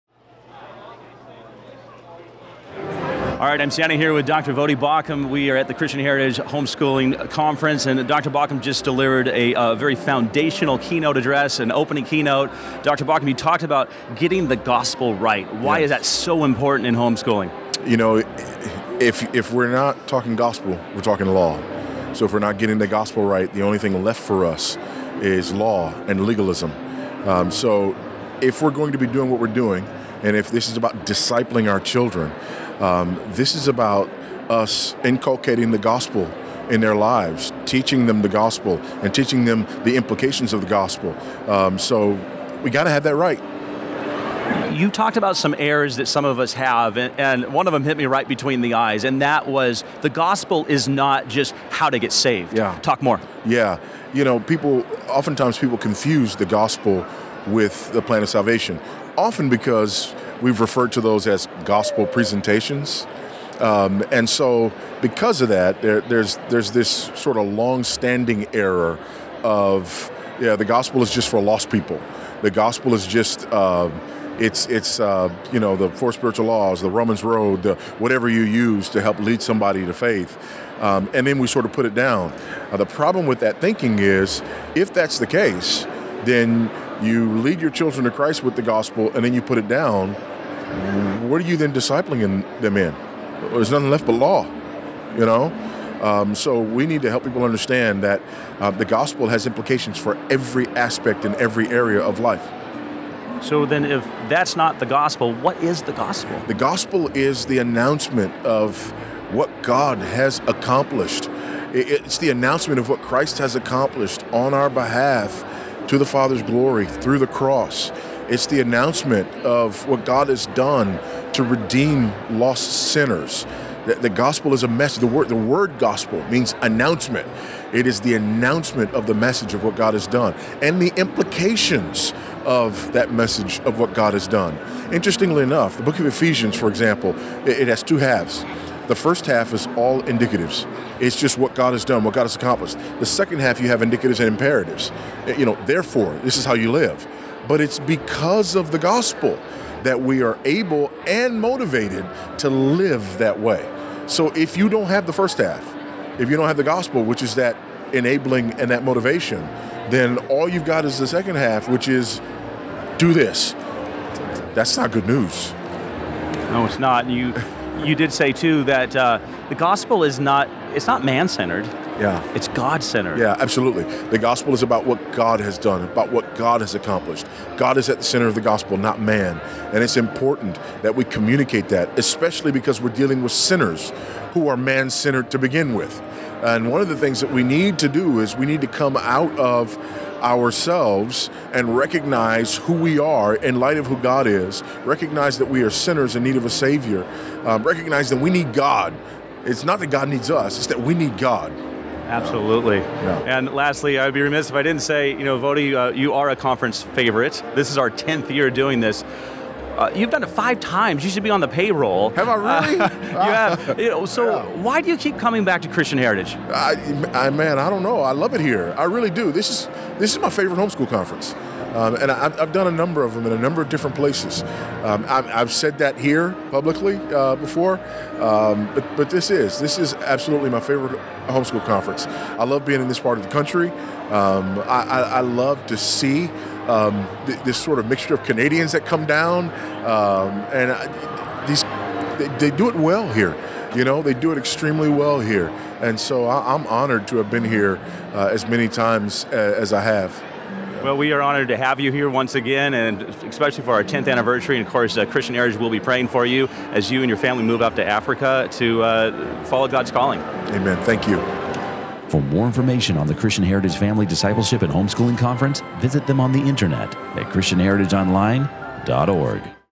Getting the Gospel Right - Voddie Baucham
"Oftentimes people confuse the Gospel with the plan of salvation -- because we've referred to those as 'Gospel Presentations.' And because of that, there's a long-standing error that the Gospel is just for lost people...and then we sort of put it down. If you lead your children to Christ with the Gospel and then you put it down, what will you disciple them in? ... There's nothing left but Law. On the contrary, the Gospel has implications for every aspect, every area of life."
- Dr. Voddie Baucham at the 2015 Christian Heritage
Family and Discipleship Conference

If that's not the Gospel, what is it then? Watch as Dr. Baucham's gives his compelling answer -- in this Christian Heritage OnAir exclusive interview.